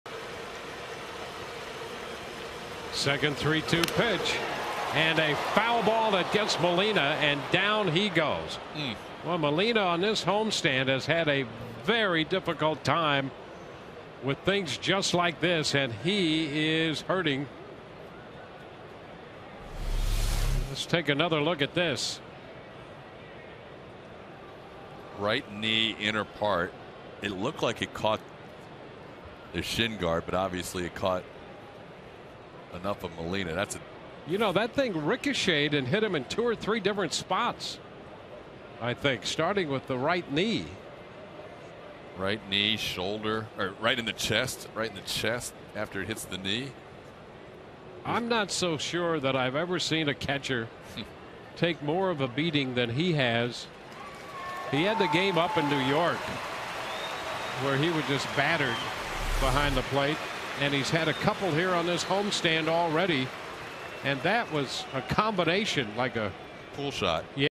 2012/06/01 Molina's injury
BAL@TB: Molina hit by foul ball in leg, stays in game